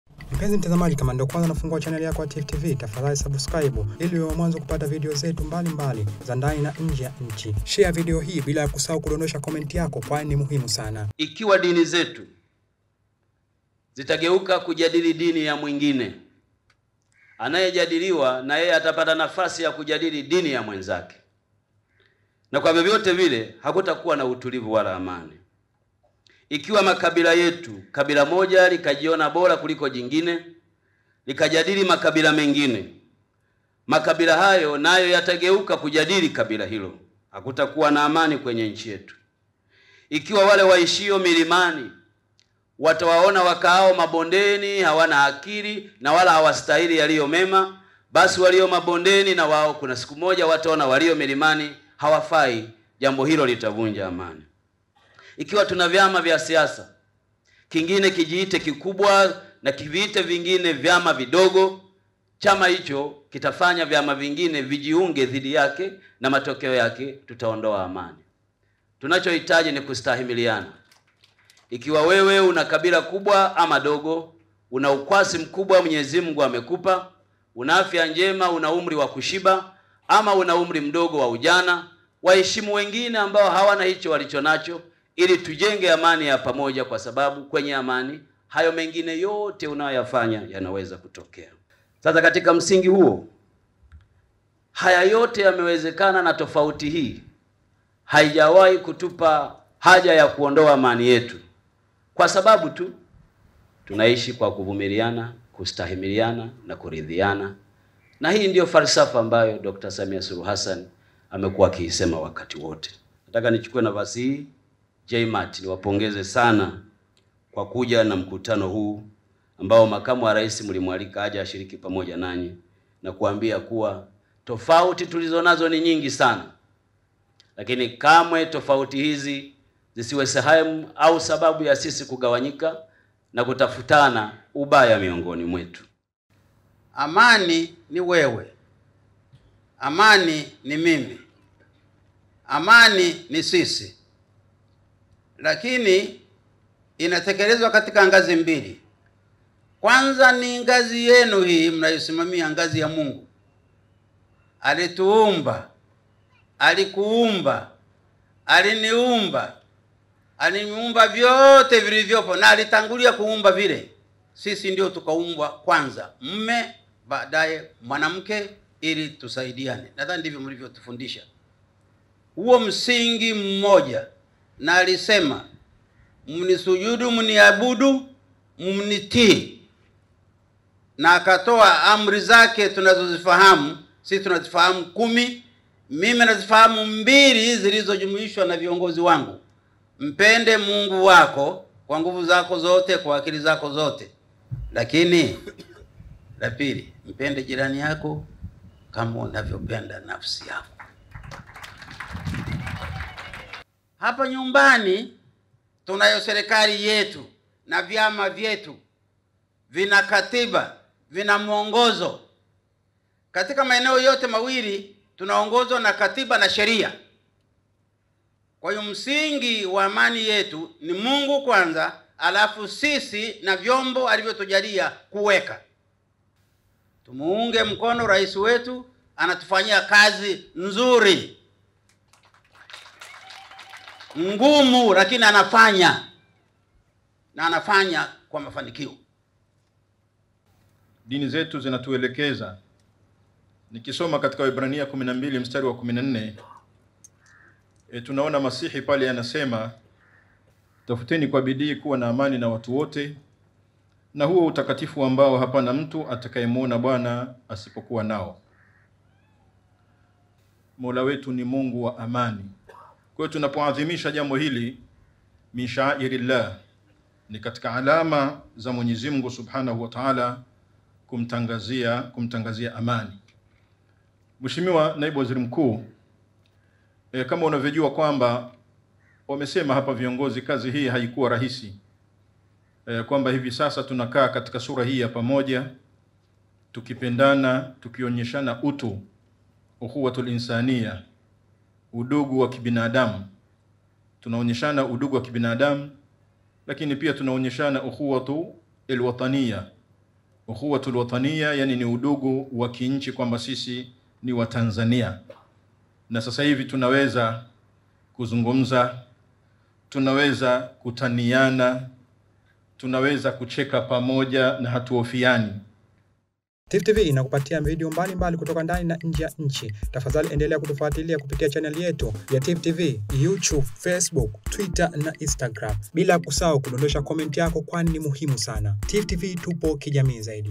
KUMEKUCHA TOFAUTI ZETU ZISIONDOE AMANI YETU – DKT BITEKO
Naibu Waziri Mkuu na Waziri wa Nishati, Mhe. Dkt. Doto Biteko amewataka Watanzania kulinda na kudumisha amani iliyopo nchini kwa kuzingatia misingi ya upendo, uvumilivu na kuheshimiana.

Dkt. Biteko ameyasema hayo leo Septemba 21, 2024 jijini Dar es salaam wakati akimwakilisha Makamu wa Rais wa Jamhuri ya Muungano wa Tanzania, Mhe. Dkt. Philip Isdory Mpango katika Kongamano la Jumuiya ya Maridhiano na Amani Tanzania (JMAT) katika Siku ya Amani Duniani inayolenga kuifanya dunia kuwa sehemu sala ya kuishi kwa kuhimiza kutunza thamani ya amani na kuzuia migogoro.

Aidha, amewaasa Watanzania kudumisha amani iliyopo nchini ili kutoa fursa kwa nchi na Watanzania kujiendelea kujiletea maendeleo.

Dkt. Biteko amesema kuwa licha ya Mwenyezi Mungu kuibariki nchi ya Tanzania kwa kuwa na eneo kubwa lenye rasilimali pamoja na watu wenye tofauti za imani, kipato na makabila bado ni lazima Watanzania kuendelea kuwa wamoja na kutunza amani kwa kuwa ni msingi wa maisha.

‘’Mungu ametuumba akatuweka katika nchi nzuri na kubwa yenye mita za mraba zaidi ya 900,000, akatupa milima, mabonde na tambarare, Mungu ametupa matabaka ya wenye navyo na wasio navyo, tofauti za umri, makabila, akatuunganisha nchi mbili Zanzibar na Tanzania Bara kuwa nchi moja haya yote yamewezekana na tofauti hizi hazijatupa haja ya kuondoa amani yetu. Tofauti tulizo nazo ni nyingi lakini zisiwe sababu za kutofautiana na kuondoa amani yetu.’’ Amesisitiza Dkt. Biteko.

Ameongeza kuwa ikiwa dini moja au kabila moja likajiona bora kuliko linguine hali hiyo itasababisha uvunjifu wa amani, na kuwa Watanzania wanahitaji kustahimiliana ili kujenga amani ya pamoja.

‘’ Amani ikitoweka wanaoumia si tu wanaosababisha itoweke bali hata wewe unayedhani haikuhusu mataifa yenye migogoro wanatumia fedha za wananchi kwenda kutafuta amani kwa kujadiliana katika nchi zingine, hivyo tuilinde nchi yetu.’’ Amebainisha Dkt. Biteko.

Fauka ya hayo, Dkt Biteko ameitaka JMAT kuwa sehemu ya kutoa elimu ya mpiga kura kwa wananchi kuhusu Uchaguzi wa Serikali za Mitaa unatarajiwa kufanyika Novemba 27, 2024 kwa kuhimiza wananchi kugombea na kuchagua viongozi wenye sifa. Aidha, amesema kuwa uchaguzi huo uwaunganishe Watanzania kwa kufanya kampeni za kistaarabu na wagombea kuelezea tofauti zao za mawazo kistaarabu.

Naye, Naibu Katibu Mkuu, Wizara ya Maendeleo ya Jamii, Jinsia, Wanawake na Makundi Maalum, Amoni Mpanjo amesema kuwa amani ni nguzo kuu ya kila jambo na ndio maana Wizara inahimiza amani ndani ya jamii. Aidha, ameipongeza JMAT kwa kuunganisha dini na watanzania na kusema Jumuiya hiyo ni mbia kwa Wizara yake na inasaidia kuwafikia Watanzania, Sambamba na kuasa jamii kuacha vitendo vya ukatili ikiwemo utekaji na ubakaji.

Mwenyekiti wa Jumuiya ya Maridhiano na Amani Tanzania (JMAT), Sheikh Alhad Mussa Salum amesema kuwa lengo la Kongamano hilo la JMAT ni kuiombea nchi ili wanaofanya vitendo vya kihalifu  kuacha na wananchi waishi kwa amani na bila hofu.

‘’ Sisi viongozi wa dini tunaamini kupitia maombi amani itaendelea na vitendo viovu vitaisha mafundisho ya dini zetu yote yanatufundisha kuhusu amani,’’ amesema Sheikh Alhad.

Mkurugenzi Idara Kuu ya Fedha, Uchumi na Utekelezaji wa  JMAT, Bw. Ayub Sanga amesema kuwa Jumuiya hiyo inashughulika kwa ajili ya amani na maridhiano na inahusisha dini zote ikiwemo Budha na Hindu na kuwa viongozi wake wanatoka katika dini mbalimbali pamoja na viongozi wa mila na desturi ili kupangana vizuri kwa ajili ya kuendelea kudumisha amani nchini.